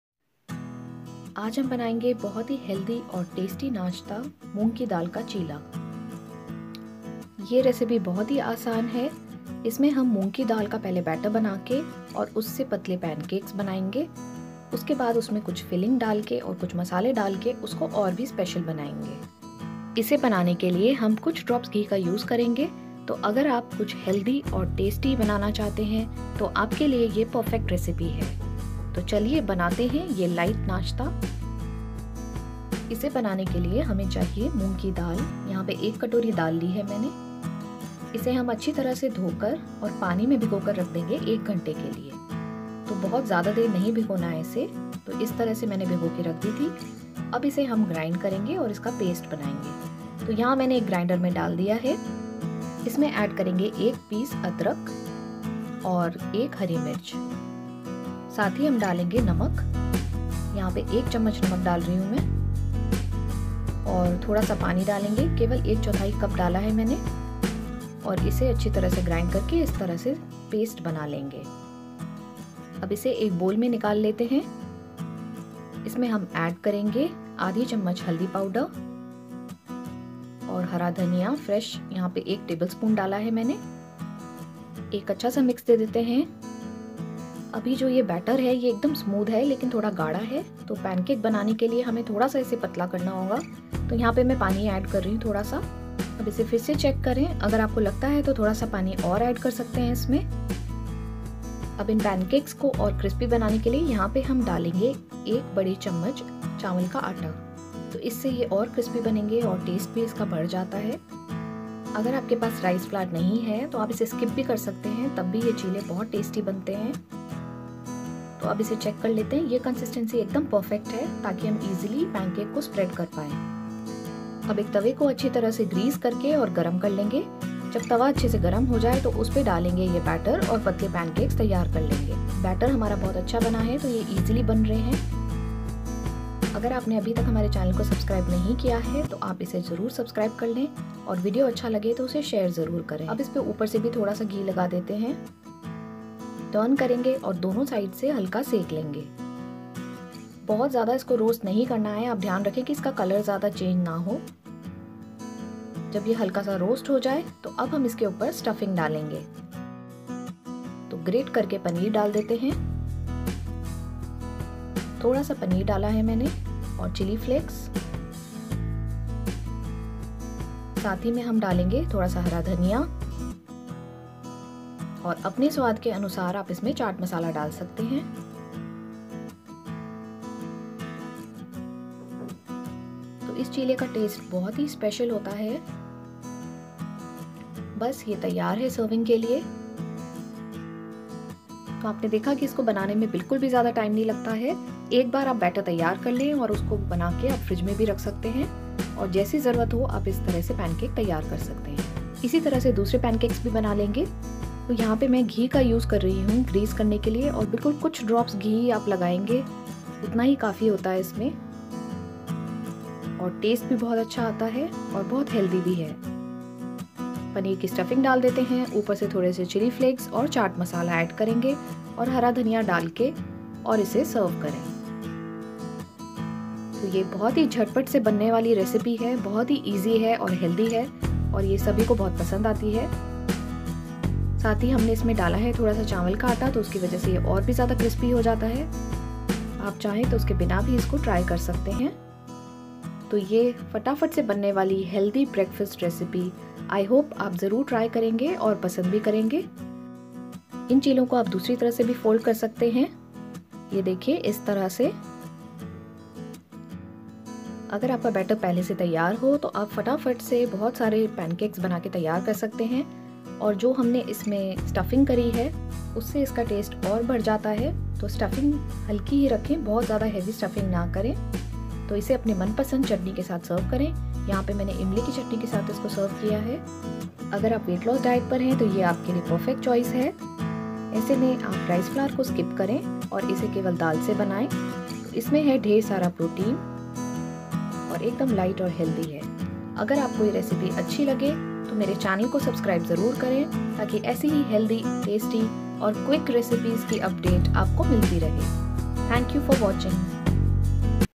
Moong Dal Chilla|मूंग दाल चीला|Tasty Chilla Recipe|Breakfast Recipe for Weight Loss|Nashta Recipe
moong dal chilla recipe,this healthy pancake is prepared using dal,paneer and some spices using just few drops of ghee,it is a very healthy and tasty breakfast recipe for weight loss,has protein rich ingredients and very easy to prepare,10 min breakfast recipe,very popular moong dal recipe,most easy healthy breakfast recipe to try like any other dal ka chilla, this chilla recipe is ideal for weight loss you can try other chilla recipes for losing weight.do follow this detailed photo video of how to make moong dal ka cheela or moong ki dal ka cheela in a quick and easy way for breakfast or as evening snacks.

To watch other recipes》

moong dal ka chilla banana bahut hi assan hai jaise besan ka chilla banate hain waise hi ise banaye nashta mein ya subah ya sham ka nashta banana bahut assan hai.moong dal mein high protein hai isme paneer stuffing dalne se aur healthy nasta banta hai jise aap jhatpat banakar taiyar kar sakte hain aur serve karein imli ki chutney ya khatti meethi chutney ke saath.moong dal ka cheela detailed video dekhe,सुबह शाम का हल्का नाश्ता जिसे एक बार बनायेंगे तो बारबार बनाने का मन करेगा।सुबह शाम का हल्का फुल्का बेहद टेस्टी नाश्ता जो झटपट बनकर तैयार हो जाये ।